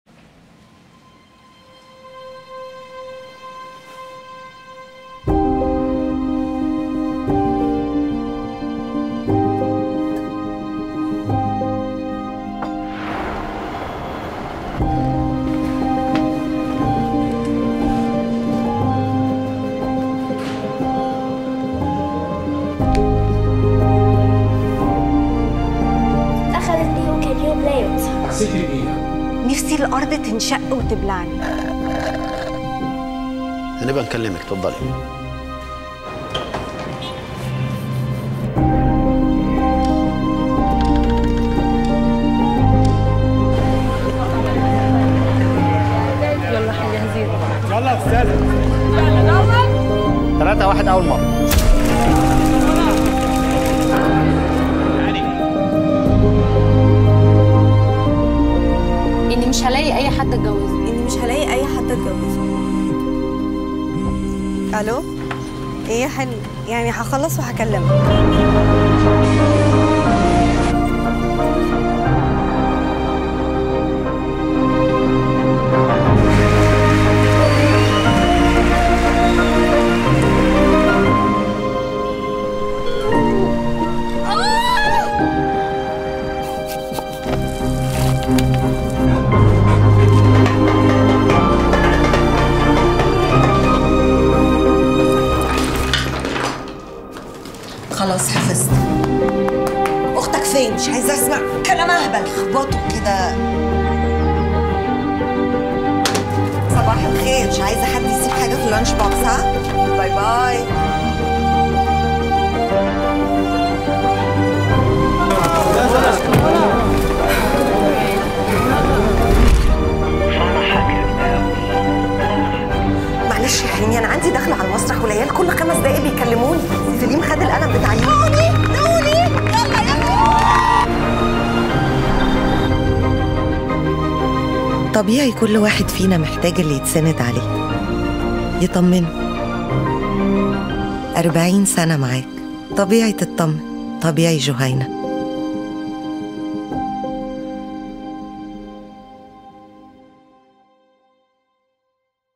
٤٠ سنة معاك. طبيعي تطمن، طبيعي جهينه. اعلان جهينه الجديد برمضان - ٤٠ سنه - منى زكى
طبيعي كل واحد فينا محتاج اللي يتسند عليه… يطمنه
٤٠ سنة معاك
طبيعي تطمن، طبيعي جهينه